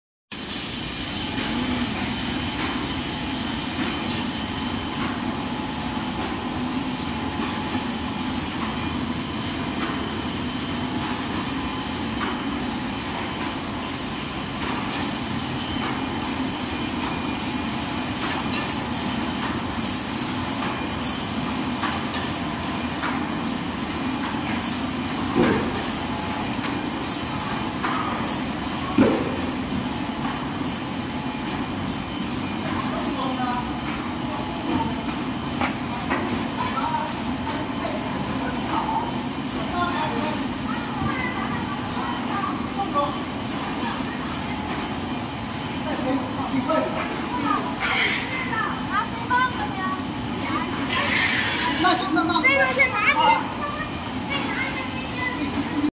Linear box straightline box one side glue paper box automatic folder gluer machine
multifuncational folding gluing machines ,also suitable for crash lock bottom ,three corner,four corner,six corner,,,, different designs boxes